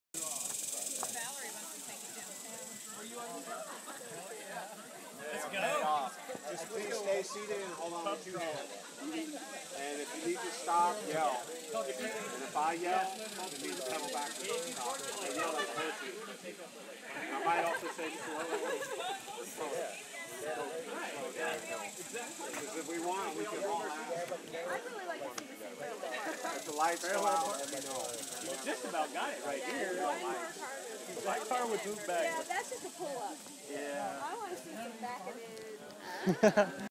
Austin Bike Zoo's snake bike
Jeremy from Austin Bike Zoo brought out the rattlesnake bike for our neighbor Lindy's birthday party (thanks to Ian). It was fantastic.